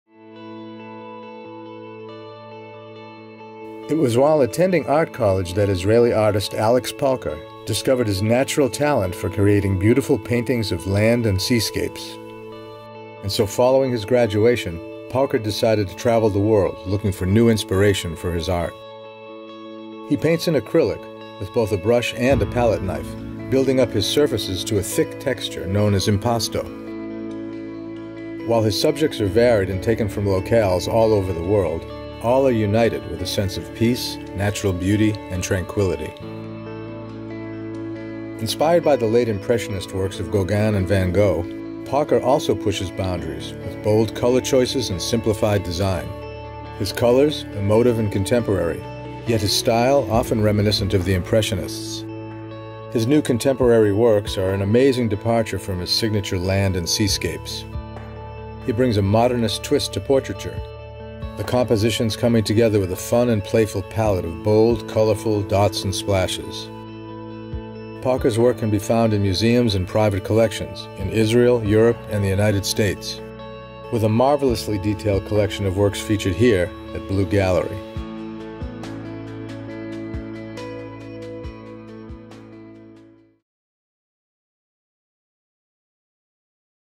Alex Pauker: Capturing the World's Beauty in Bold Colors | Blue Gallery
Welcome to the world of contemporary artist Alex Pauker, who is renowned for his distinctive and colorful landscapes and seascapes. Pauker, who was inspired by the picturesque city of Yekaterinburg, the Ural Mountains' capital, when he was born in Russia in 1974 during the Communist era, discovered his artistic potential in the Yekaterinburg College of Art.

Along with his wife Sveta Esser, Pauker ventured outside of his native country in quest of fresh sources of creative inspiration for his works. Through a student exchange program, he was able to complete his second art degree while studying in Europe, where he was enchanted by the region's breathtaking environment. He demonstrated his talent while studying by taking first place in the second International Leningrad Art Contest.

Acrylics are the medium of choice for Pauker's artwork, and he often uses a palette knife in addition to a brush to get the "impasto" thick textures. His varied subjects, drawn from all around the world, radiate tranquility, tranquillity, and natural beauty. Deeply emotive, Pauker's work is evocative of late Impressionist and Fauvist works and aims to elicit emotion through the use of vibrant, modern colors.

In his more recent work, Pauker has strayed from his trademark realistic landscapes and seascapes and adopted a modernist approach to portraiture, bringing a fun palette of abstract faces made out of bright, bold dots and splashes.

At the moment, Pauker, his wife, and their two kids reside in Berlin, Germany. His work may be seen in several private collections in Israel, Europe, and the United States, as well as being displayed in museums and galleries overseas. He continues to draw inspiration from their extensive trips around Europe.

Our Galleries:
Delray Beach